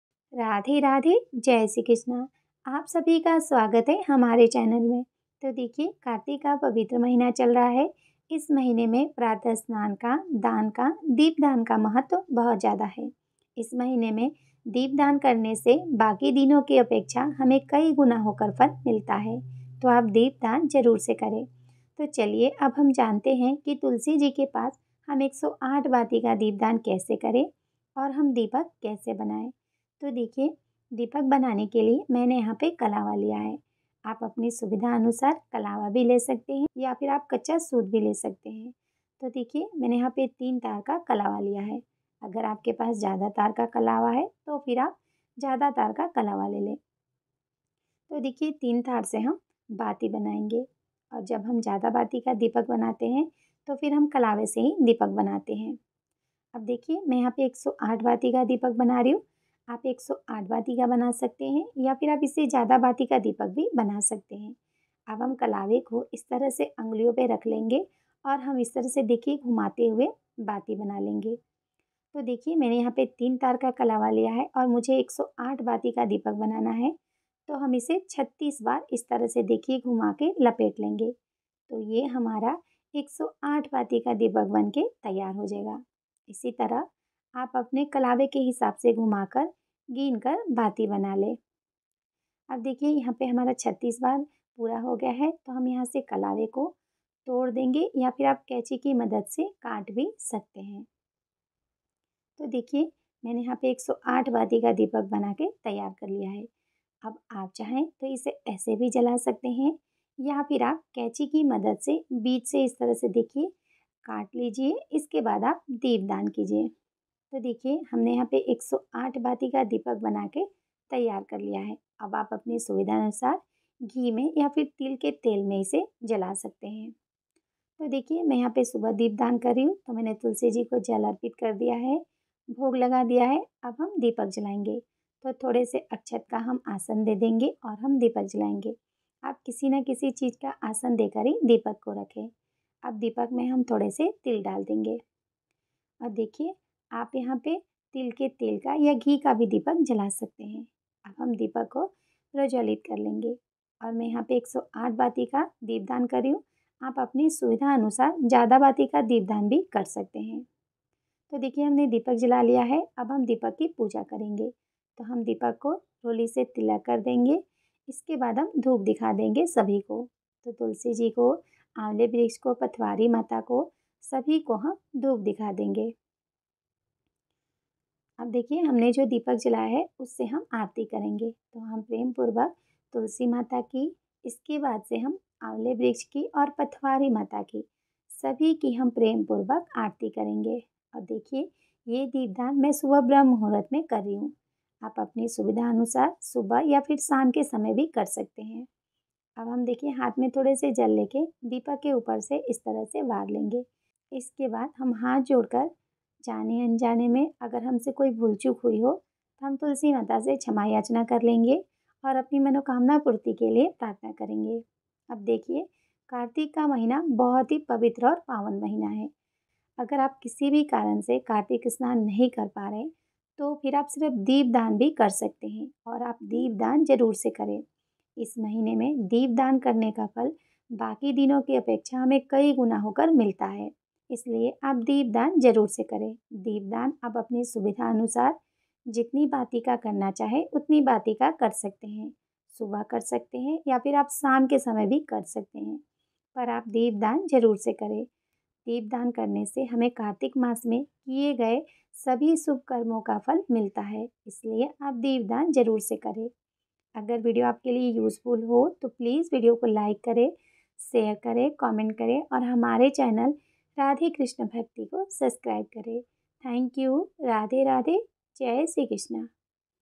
कार्तिक मास में तुलसी जी के पास 108 बाती का दीपदान कब और कैसे करें/बाती कैसे बनाएं/Tulsi Deepdan
Tulsi Mata ke pass deepdan kaise karen
108 bati ka Deepak kaise banaen
108 bati ka Deepak kab jalaya
108 badi kaise banaen